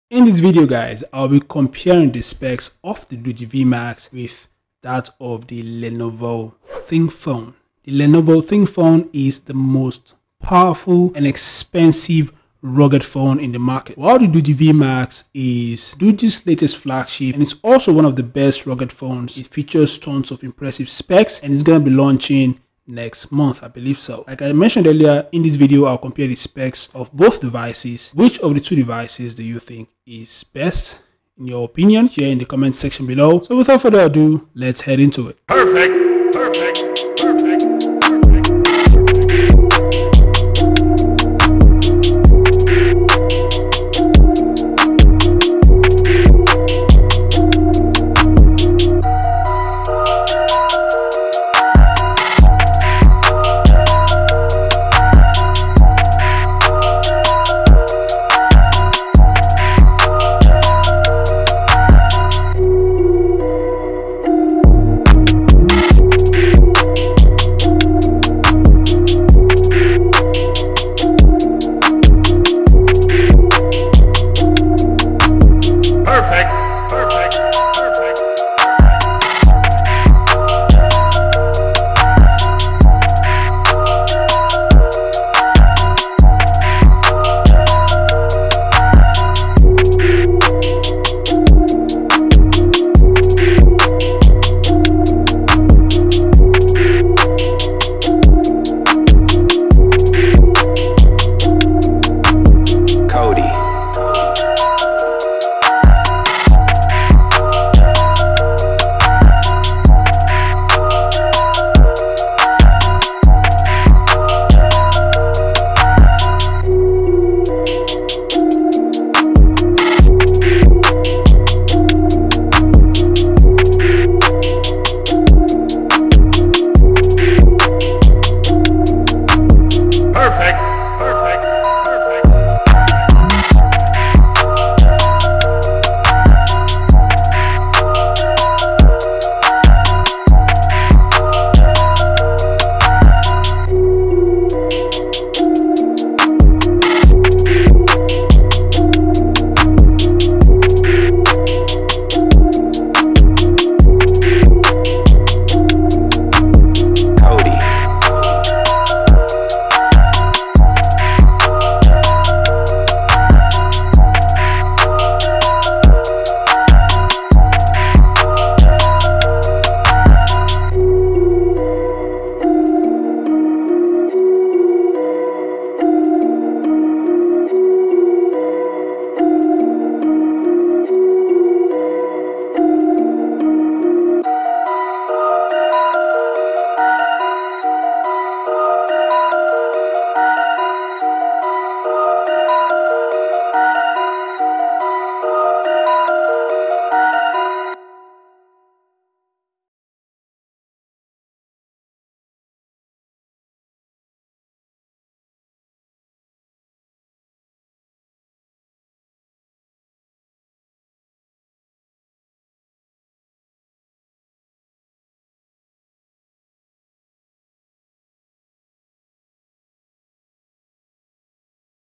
Doogee V Max vs Motorola ThinkPhone
Lenovo Thinkphone By Motorola or Motorola Thinkphone is the most expensive rugged phone in the world, it's a rugged flagship that features the Snapdragon 8+ gen 1 processor, 12gb ram, Ip68 rating, mil-std-810h compliant and tons of other cool features and specs

Doogee V Max is Doogee's latest rugged flagship, it features the Dimensity 1080 processor, 12gb ram, Ip68/69k ratings, mil-std-810h compliant and more cool features and specs, it's a lot affordable compared to the Lenovo Thinkphone by Motorola.

Turn on subtitles, if you can't understand me 🙂